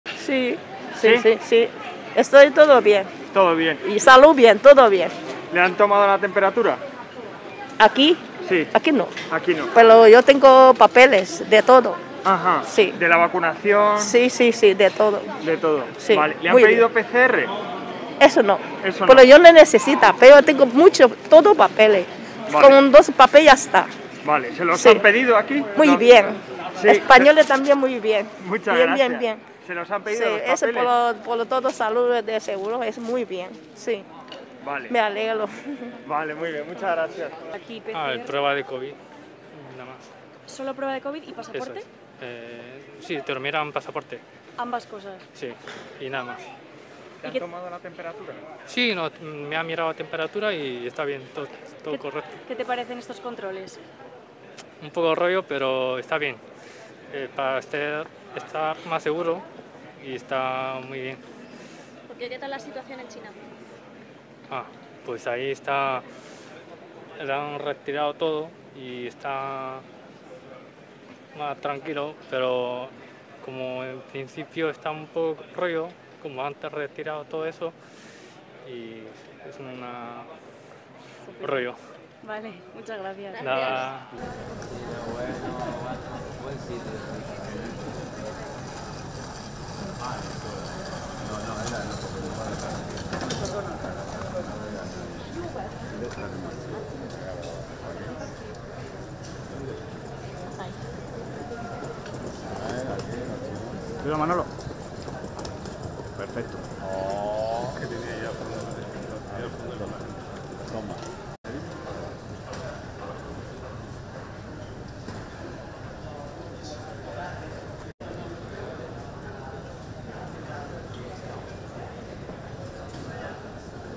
Viajeros procedentes de China explican los controles sanitarios al llegar a Madrid
Pasajeros procedentes del primer vuelo procedente de China al aeropuerto Madrid-Barajas, que han llegado el día en el que además de exigir a los pasajeros que presenten una prueba negativa de Covid también tienen que aportar Certificado Covid o equivalente, han explicado cuáles han sido los controles sanitarios al aterrizar en Madrid.